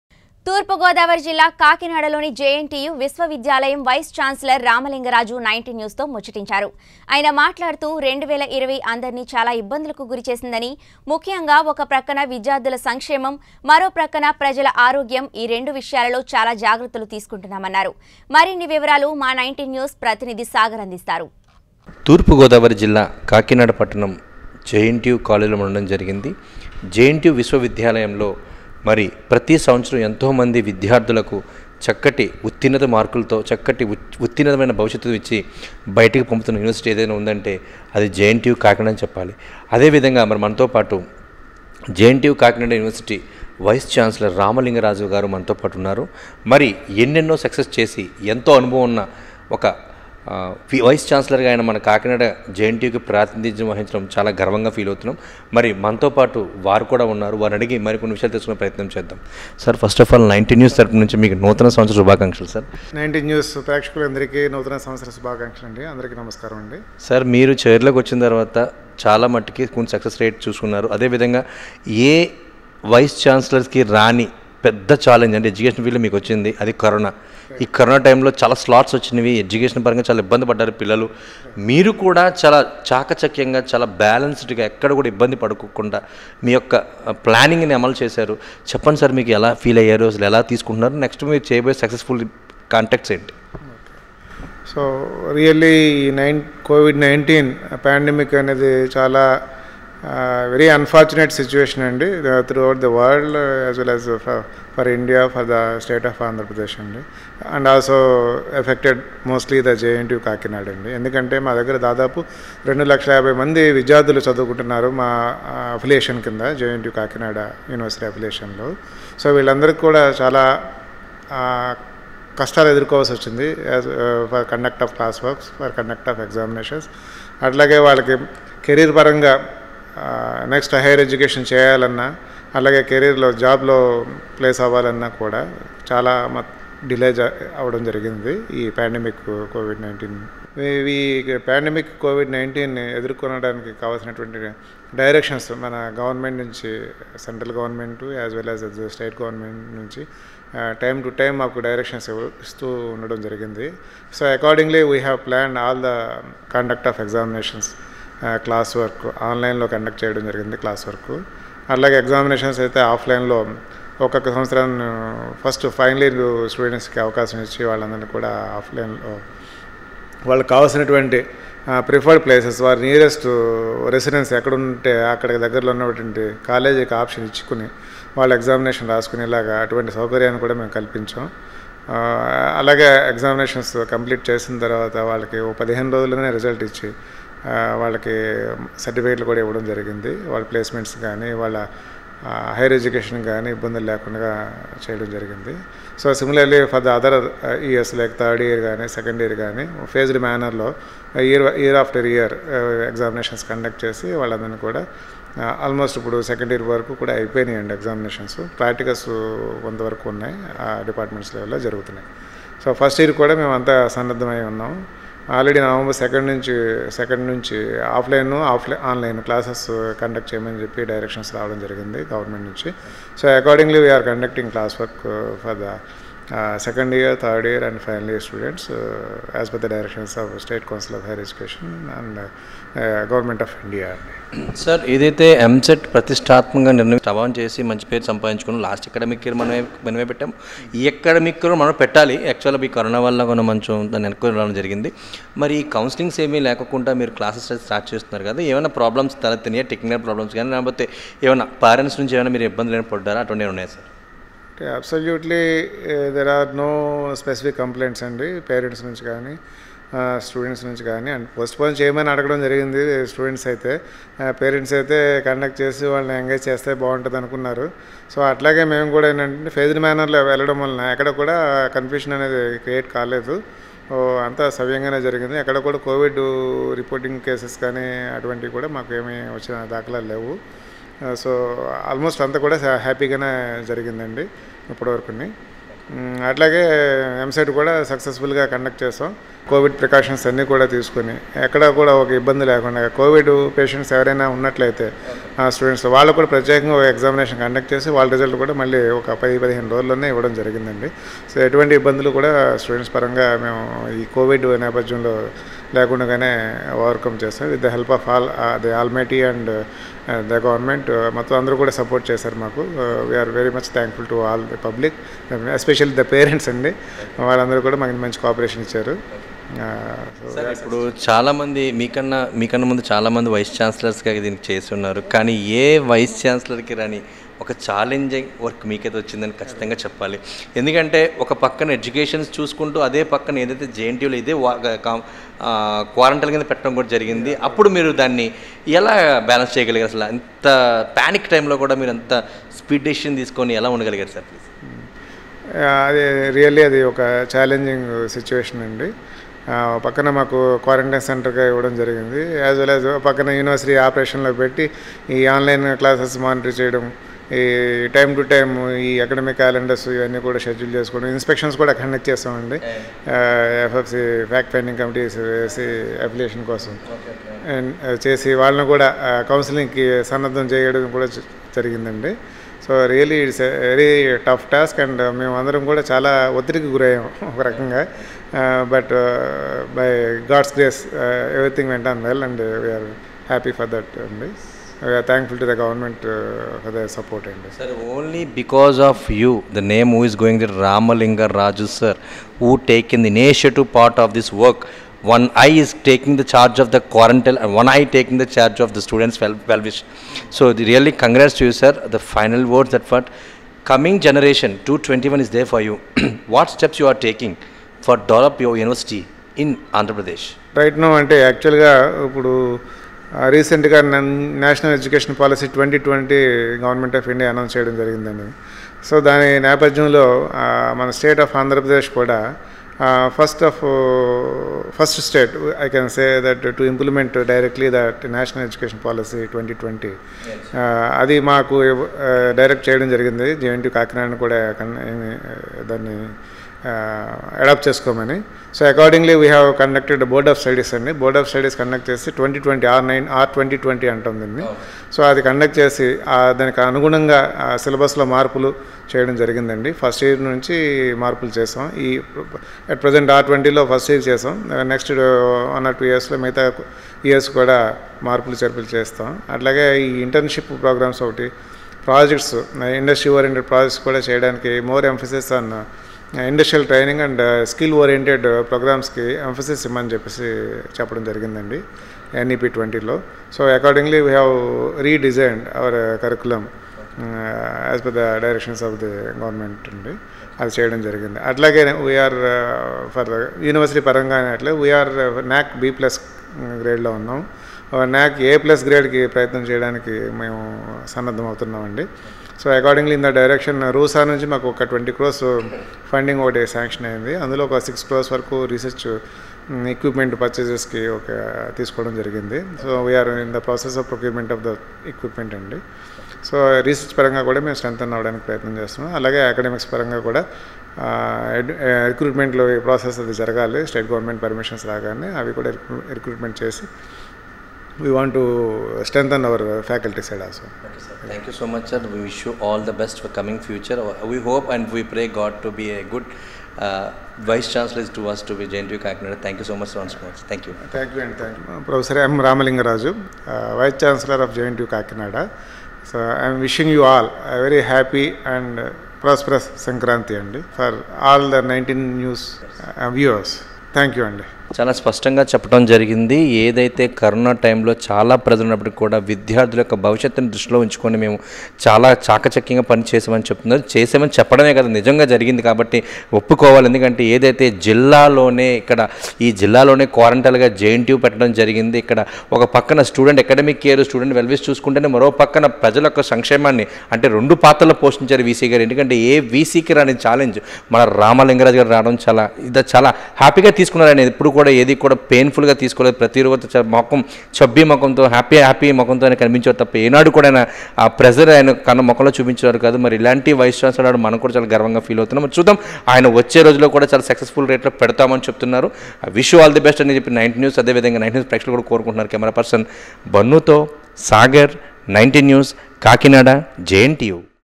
కాకినాడ జేఎన్టీయూ విశ్వవిద్యాలయం వైస్ ఛాన్సలర్ రామలింగరాజుతో 19 న్యూస్ ముఖాముఖి తూర్ప గోదావరి జిల్లా
కాకినాడ జేఎన్టీయూ విశ్వవిద్యాలయం వైస్ ఛాన్సలర్ రామలింగరాజుతో 19 న్యూస్ ముఖాముఖి తూర్పు గోదావరి జిల్లా | 19 Newstelugu